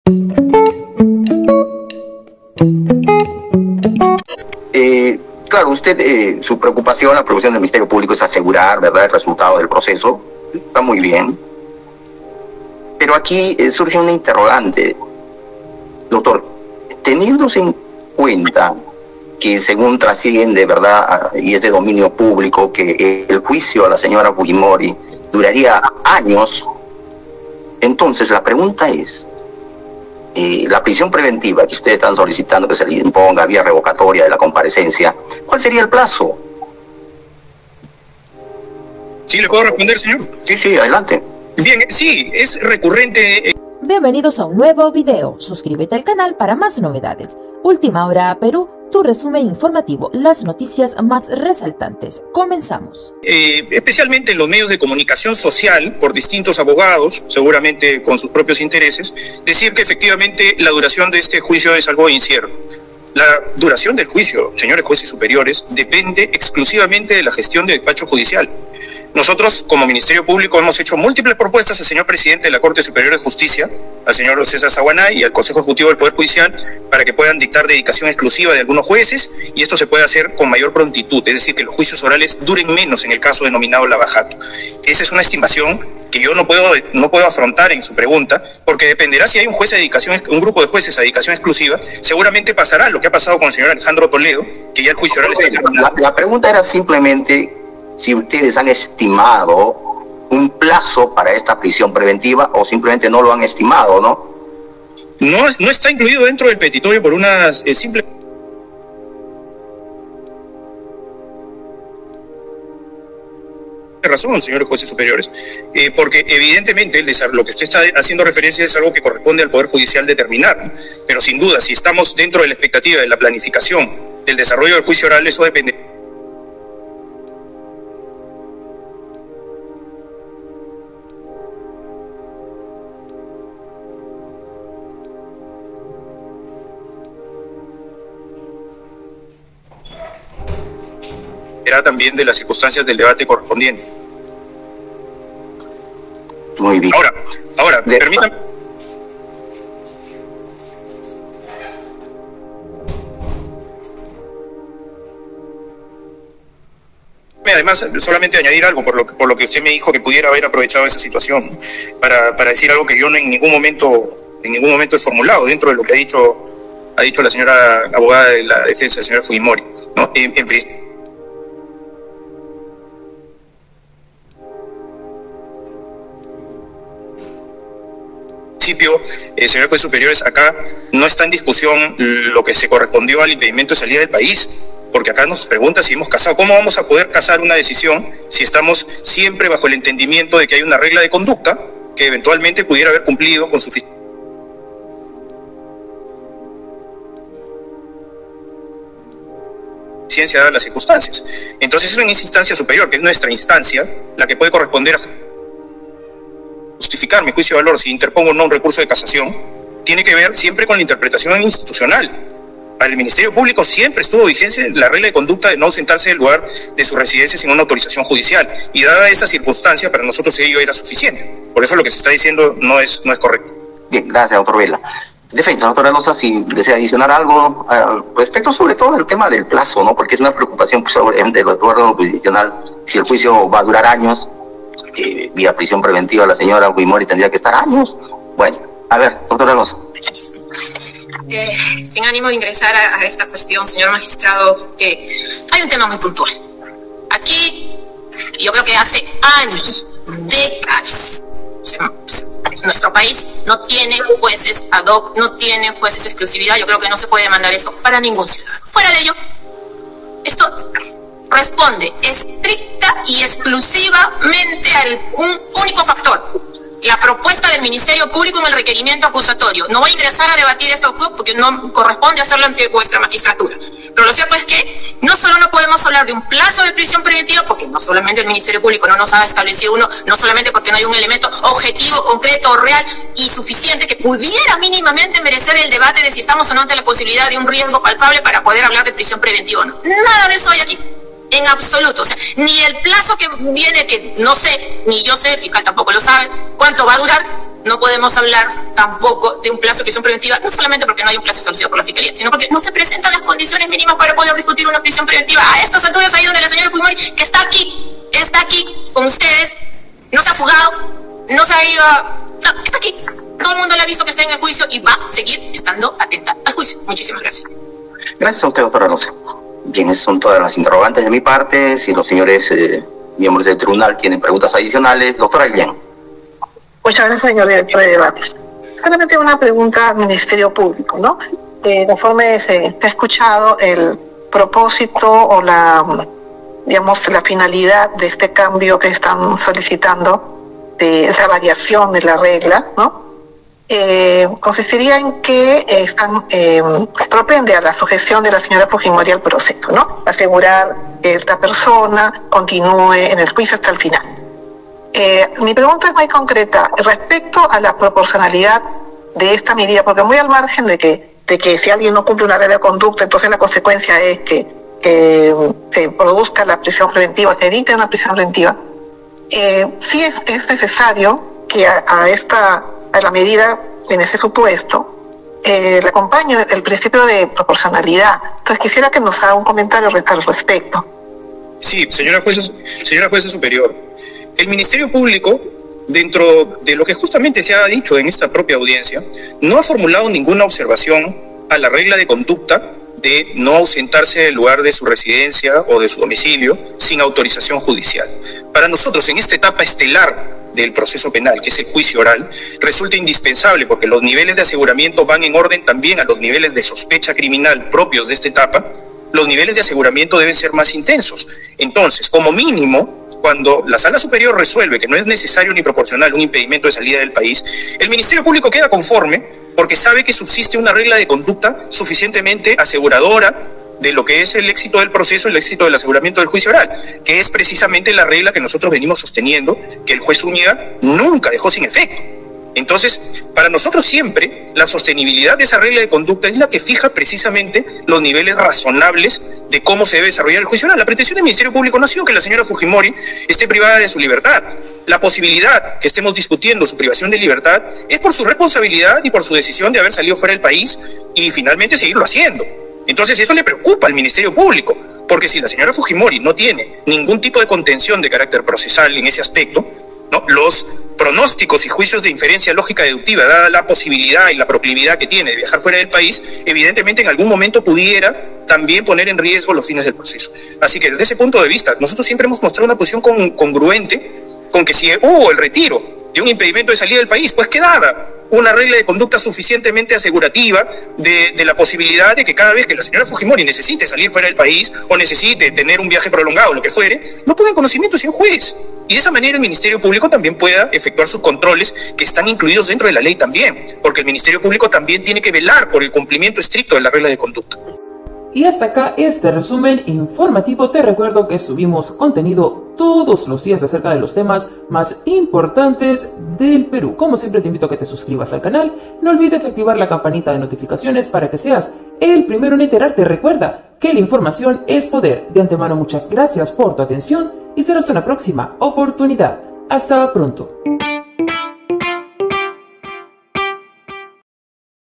BRAVO! VALIENTE JUEZ MANDA A CALLAR AL FISCAL RAFAEL VELA Y LO HACE LL0RAR
MUSICA SIN COPYRIGHT PARA TUS VLOGS Y VIDEOS EN GENERAL EN   @Pileusmusic